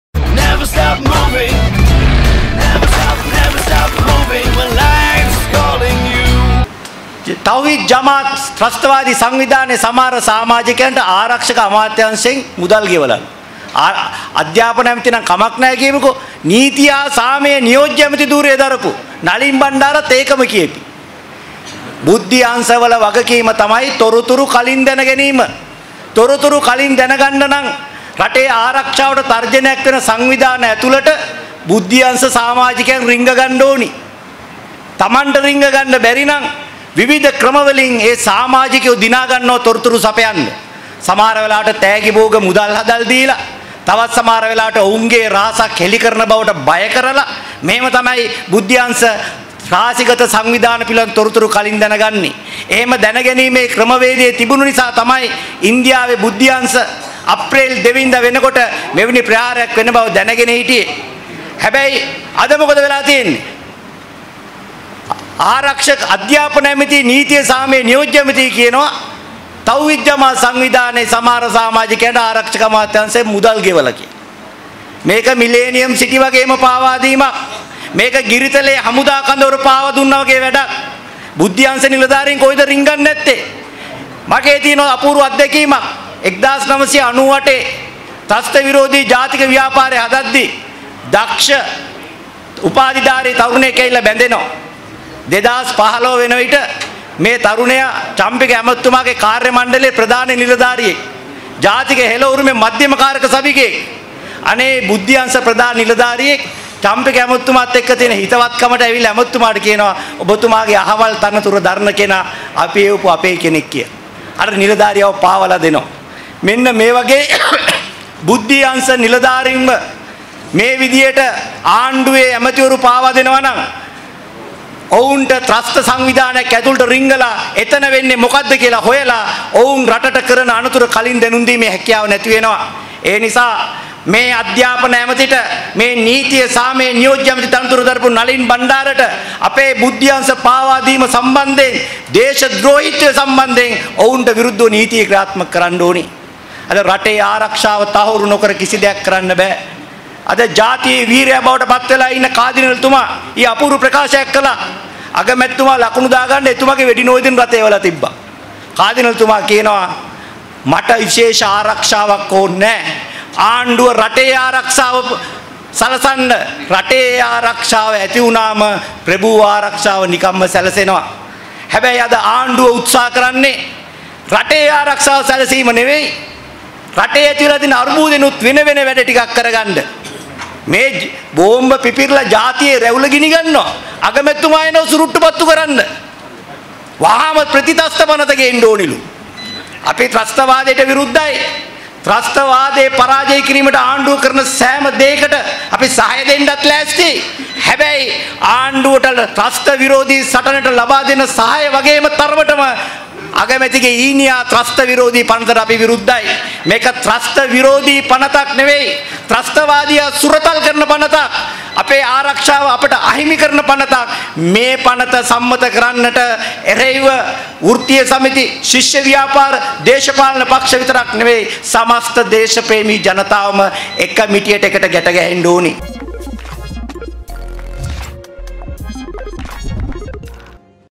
අකිල විරාජ් සහ නලීන් බණ්ඩාරට නීතිය ක්‍රියාත්මක කළ යුතුයි බුද්ධි අංශ පාවාදීම නිසා | Breaking.lk
අකිල විරාජ් සහ නලීන් බණ්ඩාරට නීතිය
ක්‍රියාත්මක කළ යුතුයි බුද්ධි අංශ පාවාදීම නිසා